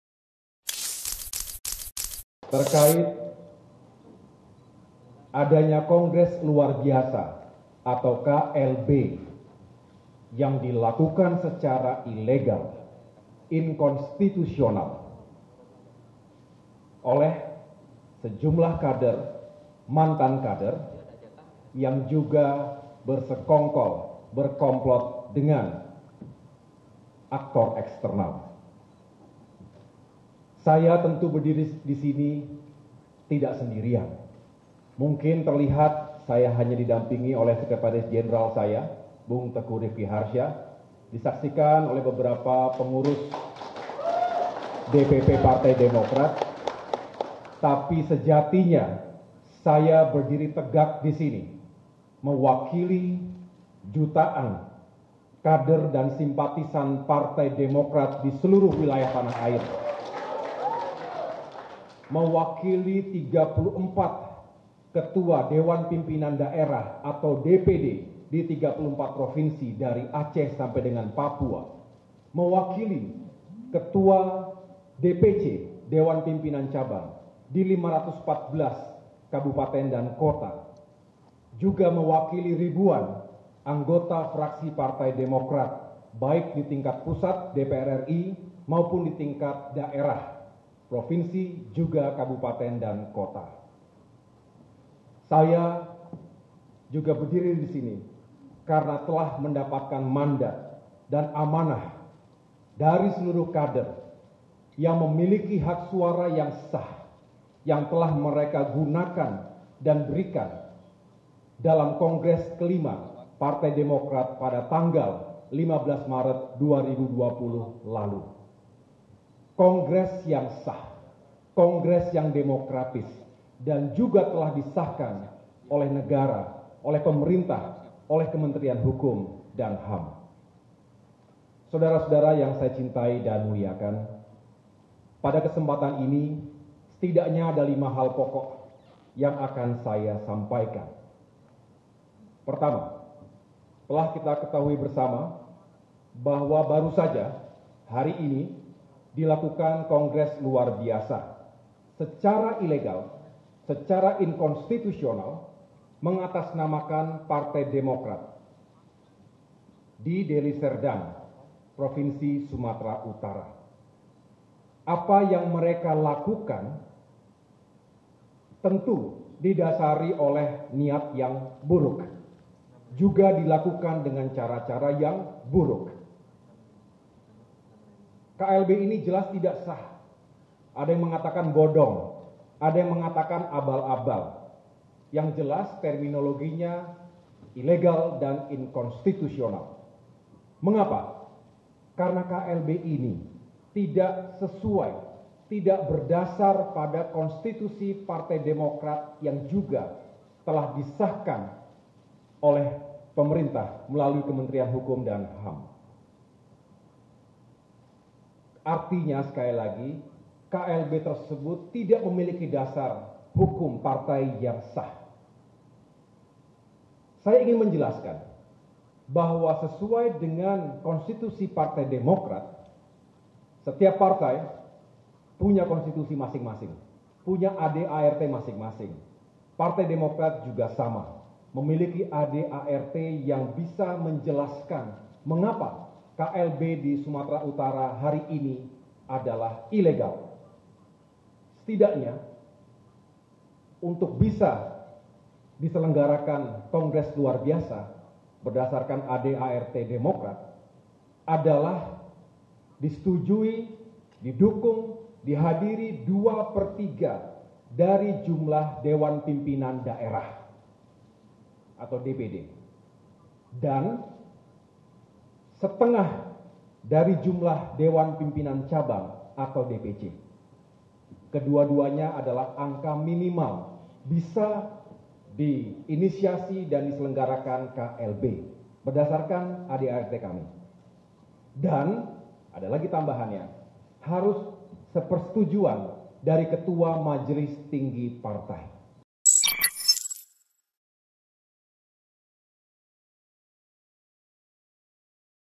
AHY Sebut KLB Demokrat di Deli Serdang Ilegal
Ketua Umum Partai Demokrat Agus Harimurti Yudhoyono alias AHY menyebut jika pelaksaanan Kongres Luar Biasa yang digelar di Deli Serdang Sumatra Utara merupakan kegiatan ilegal. AHY juga menyebut jika KLB yang mengungkuhkan  Moeldoko sebagai ketua umum adalah tindakan yang melanggar konstitusi.

"Terkait adanya kongres luar biasa atau KLB yang dilakukan secara ilegal, inkonstitusional, oleh sejumlah kader, mantan kader yang juga bersekongkol dan berkomplot dengan kader eksternal," kata AHY.

Dia mengatakan jika kegiatan KLB di Deli Serdang itu tidak memiliki dasar hukum yang sah sesuai AD/RT Partai Demokrat. Polri juga memastikan tak mengeluarkan izin penyelenggaraan Kongres Luar Biasa (KLB) Partai Demokrat di tengah pandemi covid-19. Kegiatan itu berlangsung di The Hill Hotel and Resort Sibolangit, Kabupaten Deli Serdang, Sumatra Utara (Sumut).

Dalam sidang pleno KLB Partai Demokrat, Moeldoko didaulat menjadi Ketua Umum Partai Demokrat periode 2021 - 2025 menggantikan Agus Harimurti Yudhoyono. Selain itu, sidang pleno KLB Partai Demokrat juga memutuskan secara aklamasi bahwa Marzuki Alie menjadi Ketua Dewan Pembina Partai Demokrat. Keputusan itu dibacakan oleh pemimpin sidang pleno KLB Partai Demokrat Jhoni Allen Marbun.